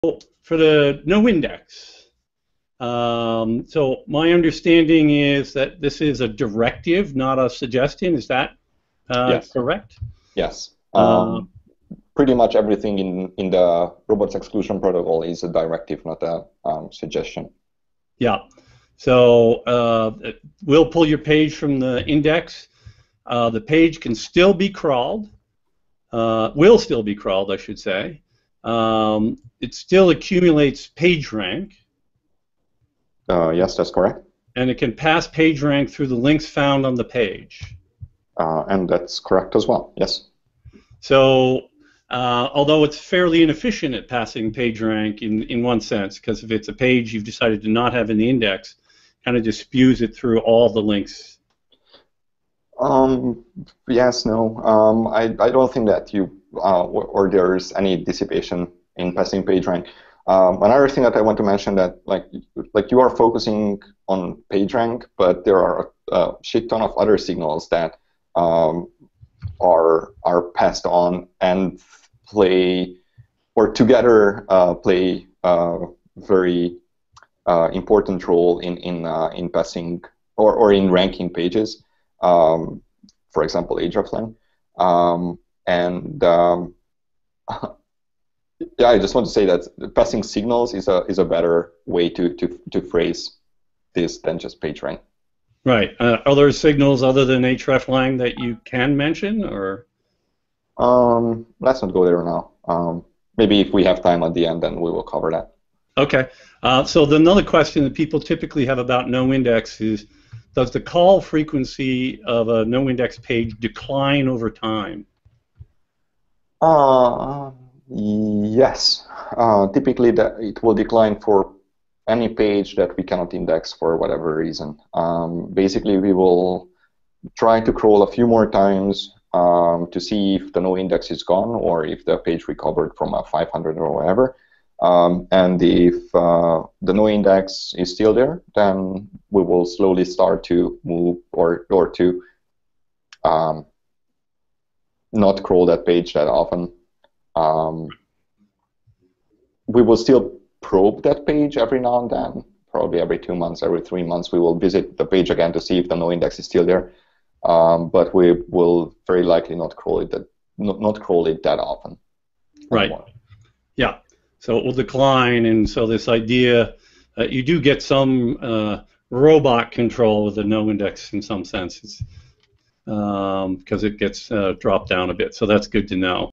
Gary Illyes on Noindex Tags - Stone Temple Consulting Virtual Keynote 2
Google Webmaster Trends Analyst Gary Illyes talks with Stone Temple Consulting CEO Eric Enge about how Google treats the noindex tag for SEO in this excerpt from our Virtual Keynote on SEO Tags.

Need to apply this to your business? Talk to us!